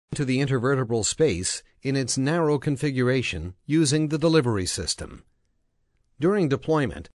Disc-O-Tech - video animation of spinal procedure, produced by Virtual Point
Medical Training guide of spinal minimally invasive procedure. Produced by "Virtual Point" 3D medical animation spinal procedure